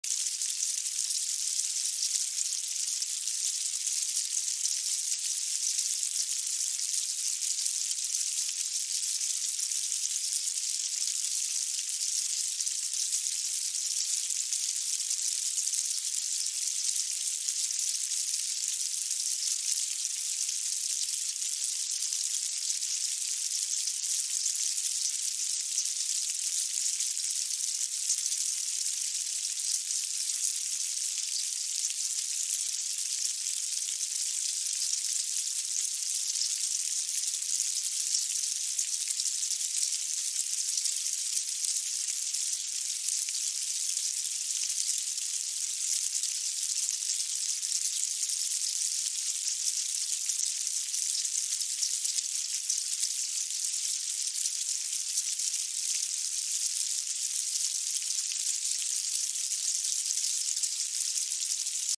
Música com som de Cachoeira para relaxar.
Hora de fazer uma pausa no seu dia e se equilibrar para novas tomadas de decisão.
Sons e imagens da natureza te reconectam contigo.